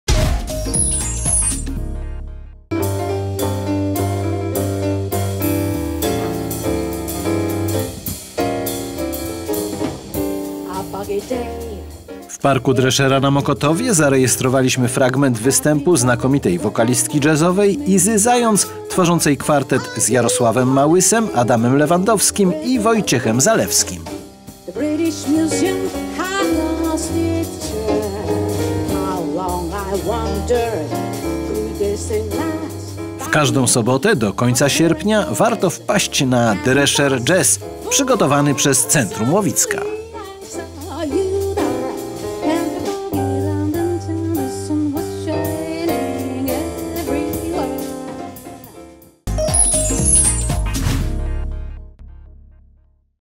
Koncert jazzowy Izy Zając Quartet w Parku Dreszera, w ramach Dreszer Jazz
W Parku Dreszera na Mokotowie w każdą sobotę wakacji odbywa się  koncert jazzowy w ramach Dreszer Jazz . Tym razem Iza Zając Quartet